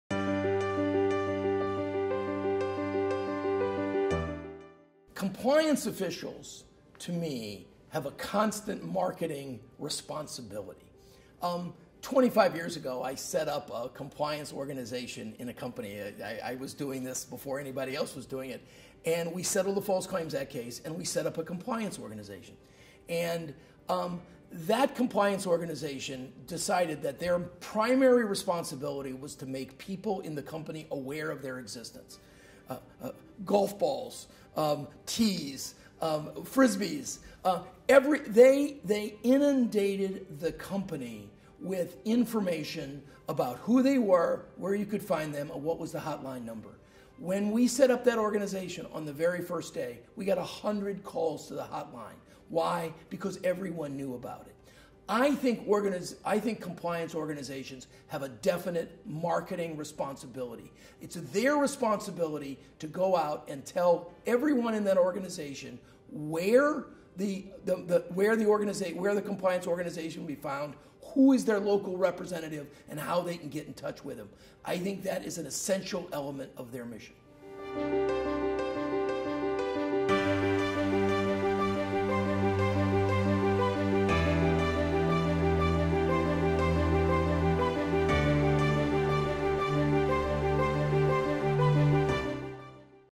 How can compliance programs encourage employees to come forward internally?
Jack Boese, Of Counsel, Fried, Frank, Harris, Shriver & Jacobson LLP discusses how compliance programs can encourage employees to come forward internally.